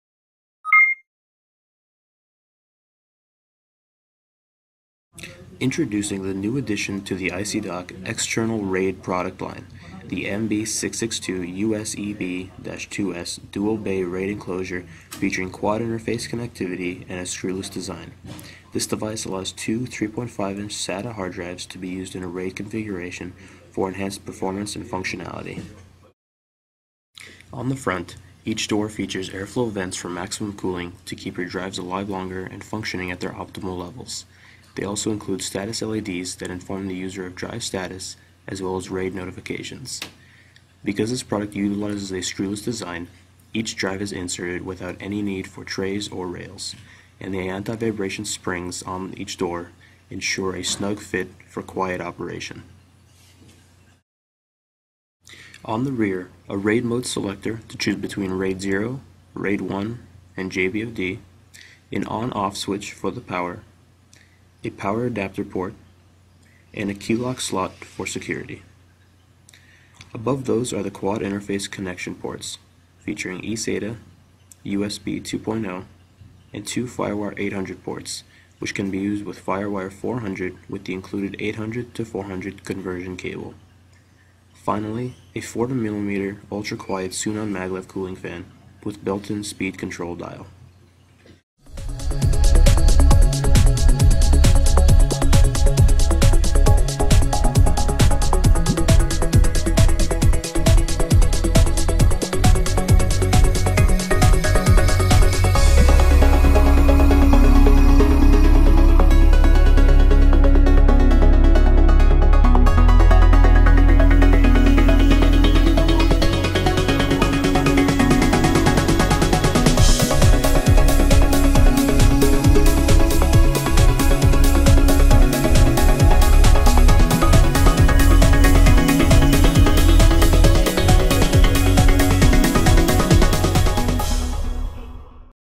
3.5吋SATA雙層式硬碟外接盒 | ICY DOCK ICYRaid 【MB662USEB-2S-1】
MB662USEB-2S最大的改變則是在於傳輸介面上的不同：USB2.0, eSATA, Firewire 800與Firewire 400，並適用於Windows, Mac甚至Linux作業系統，讓習慣不同作業系統的消費者可以恣意選擇所喜愛的傳輸介面。另外MB662USEB-2S單槽支援最高2TB的硬碟，並且新增了三種RAID控制功能；JBOD、RAID0、RAID1，不論是空間合併(最高4TB)、同步寫入或者鏡射儲存都非常輕鬆，只要重新設定後再次開機就可以直接切換所需要的模式，相當簡單方便！